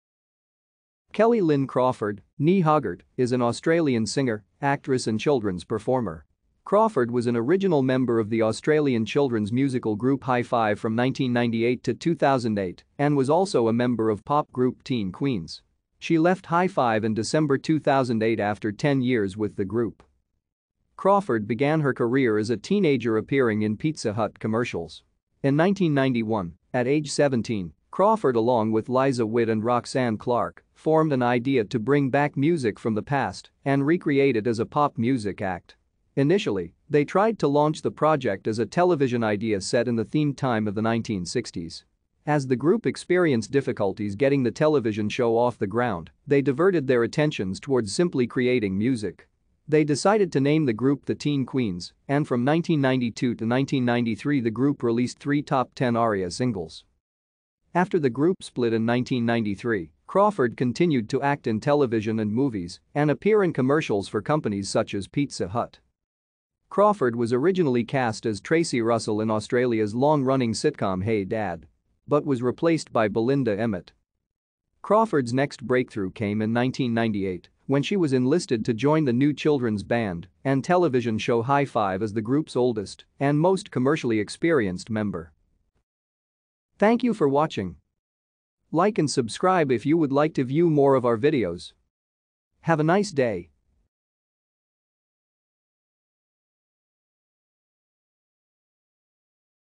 Kellie Crawford
Kellie Lynn Crawford, née Hoggart, is an Australian singer, actress and children's performer. Crawford was an original member of the Australian children's musical group Hi-5 from 1998 to 2008 and was also a member of pop group Teen Queens. She left Hi-5 in December 2008 after ten years with the group.